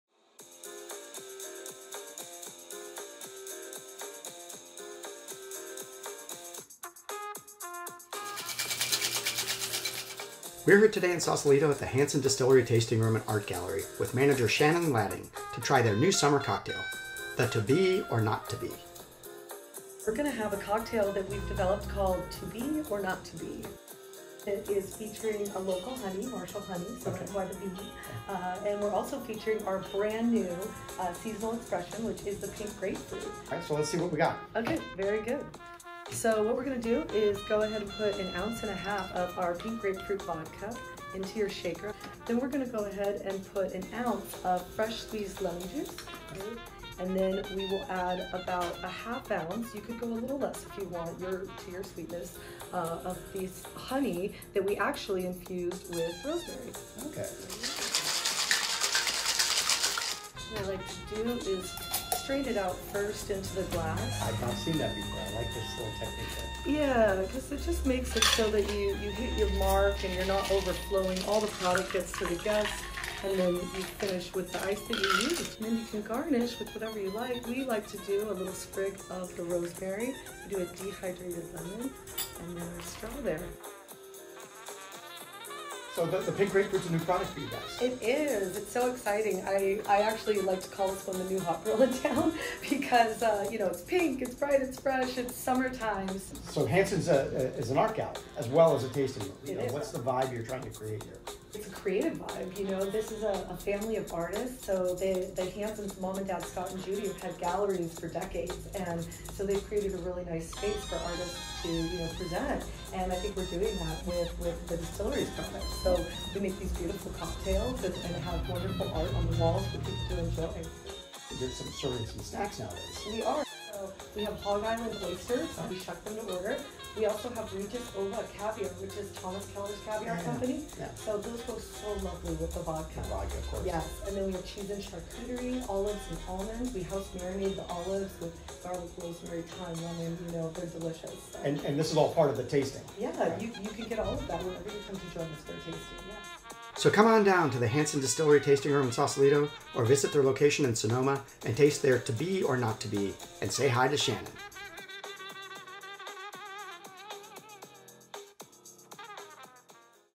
Meet Your Maker - Hanson Distillery
To be or not to be? That is the question. And it also happens to be the cocktail that Shannon Latting (Hanson Distillery's tasting room manager) shows Jeff Burkhart how to make today. The "To Bee Or Not To Bee" is made with Hanson's Pink Grapefruit Vodka, lemon juice, and rosemary-infused honey. Needless to say, this summer cocktail is un-BEE-lievably tasty. Cheers!